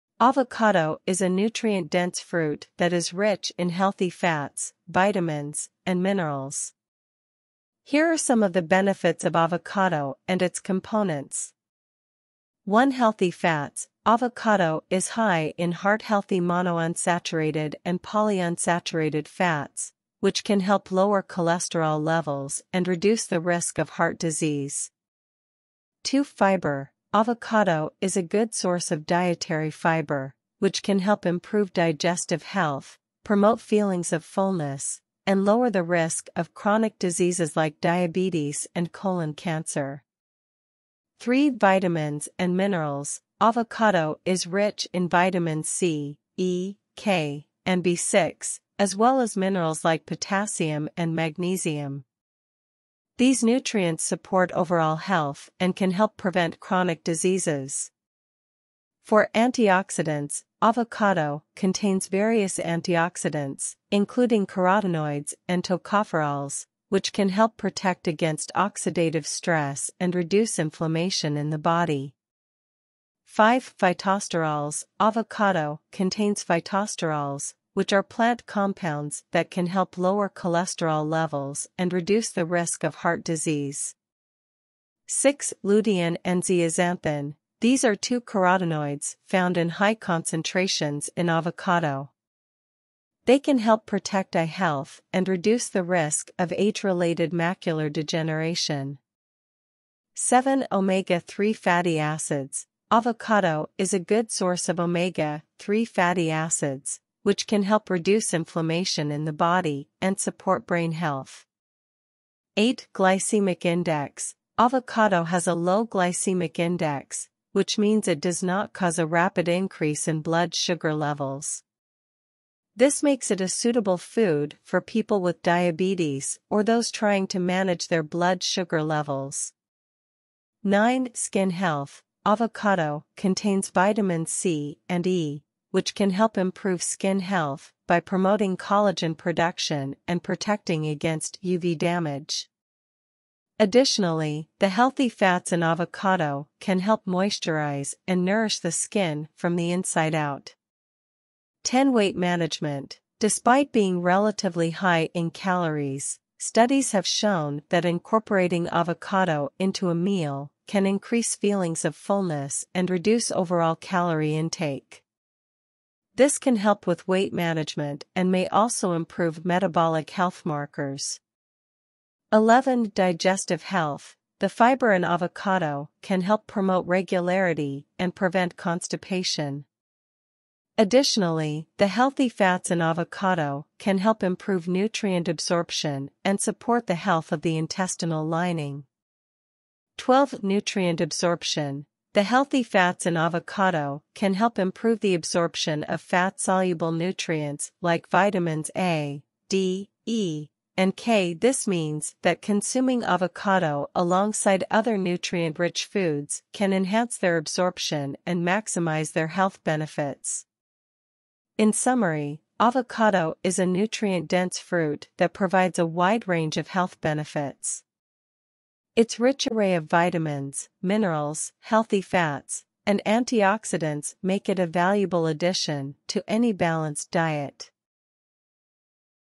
Benefits of Avocado
From Heart Health to Weight Management: The Many Benefits of Avocado
Avocado: The Nutrient-Dense Fruit with Surprising Benefits
The Health Benefits of Avocado: Why You Should Add it to Your Diet

Avocado is a nutrient-dense fruit that is rich in healthy fats, vitamins, and
minerals. Here are some of the benefits of avocado and its components:

Healthy fats: Avocado is high in heart-healthy monounsaturated and polyunsaturated fats, which can help lower cholesterol levels and reduce the risk of heart disease.

Fiber: Avocado is a good source of dietary fiber, which can help improve digestive health, promote feelings of fullness, and lower the risk of chronic diseases like diabetes and colon cancer.

Vitamins and minerals: Avocado is rich in vitamins C, E, K, and B6, as well as minerals like potassium and magnesium. These nutrients support overall health and can help prevent chronic diseases.

Antioxidants: Avocado contains various antioxidants, including carotenoids and tocopherols, which can help protect against oxidative stress and reduce inflammation in the body.

Phytosterols: Avocado contains phytosterols, which are plant compounds that can help lower cholesterol levels and reduce the risk of heart disease.

Lutein and zeaxanthin: These are two carotenoids found in high concentrations in avocado. They can help protect eye health and reduce the risk of age-related macular degeneration.

Overall, avocado is a nutritious and versatile fruit that can provide numerous health benefits when consumed as part of a balanced diet.